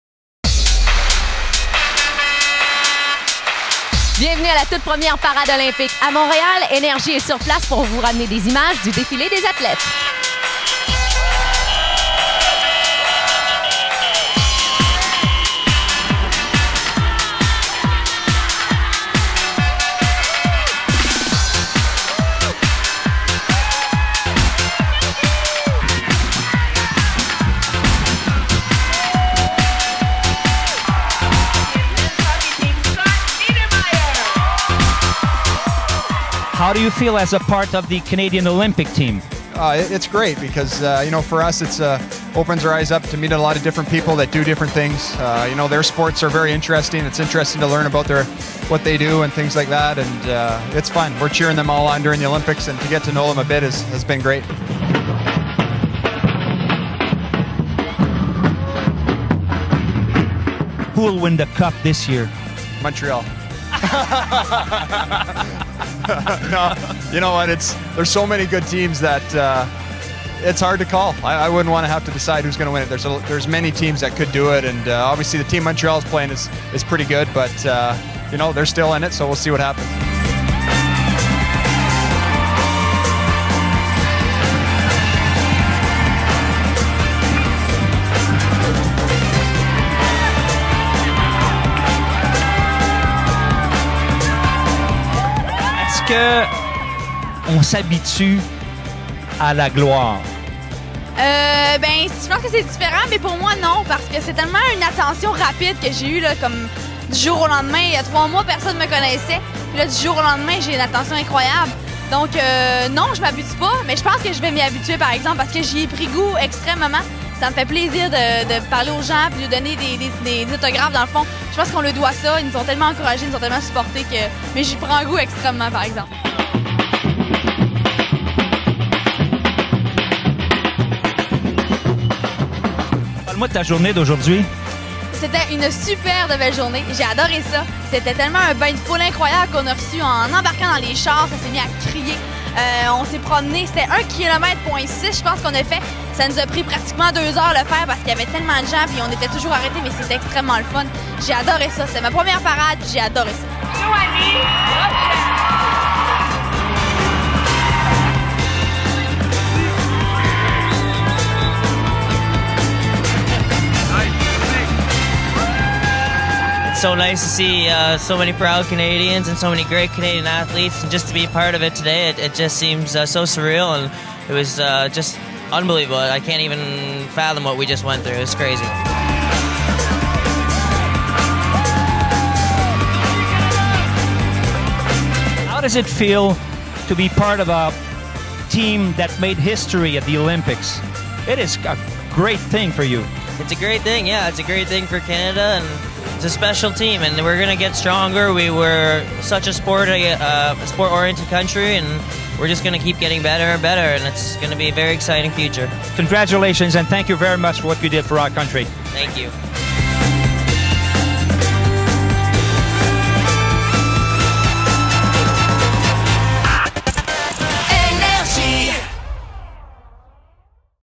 Défilé des athlètes Olympiques et Paralympiques du Canada
NRJ était sur place et vous fait revivre les moments fort du premier défilé des athlètes Olympiques et Paralympiques du Canada